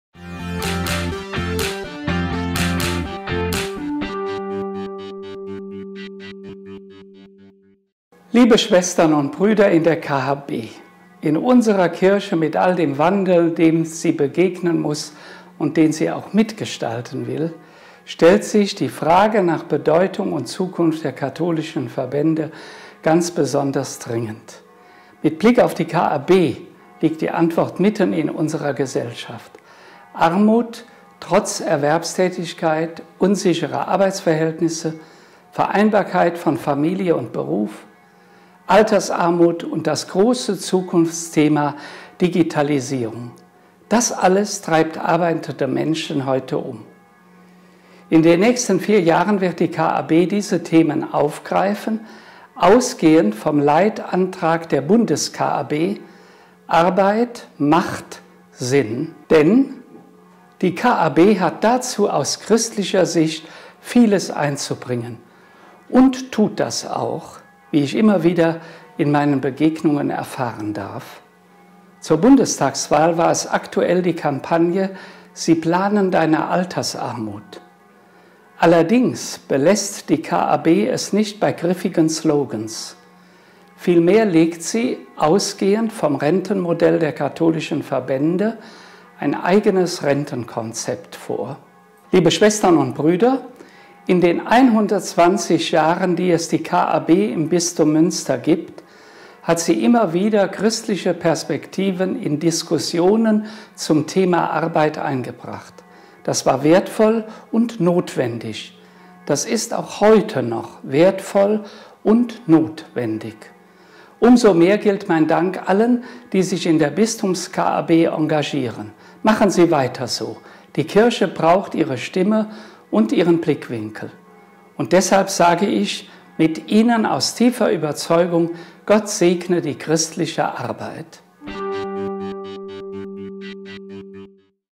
Botschaft Bischof Dr. Felix Genn zum KAB-Diözesantag 2017
Mit einer Videobotschaft grüßt Bischof Dr. Felix Genn die Delegierten des Diözesantages der KAB. Er bestärkt die KAB in ihrem Engagement in mitten von Gesellschaft und Kirche. „Die Kirche braucht Sie und ihre Blickrichtung“, betont der Bischof.